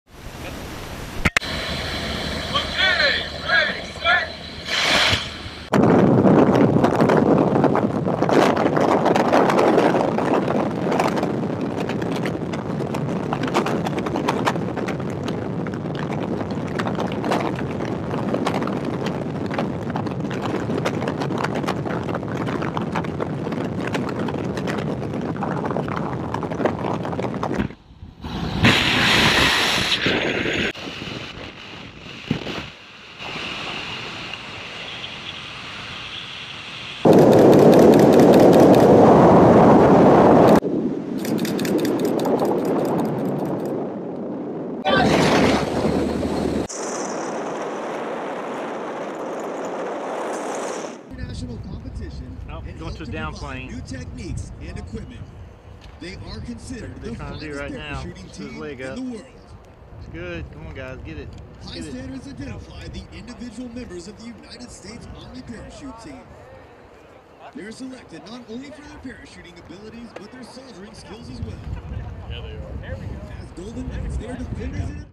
Army Golden Knights Parachute Team
United States Army Golden Knights parachute team in 2015. (Footage courtesy Michael Battise, U.S. Army)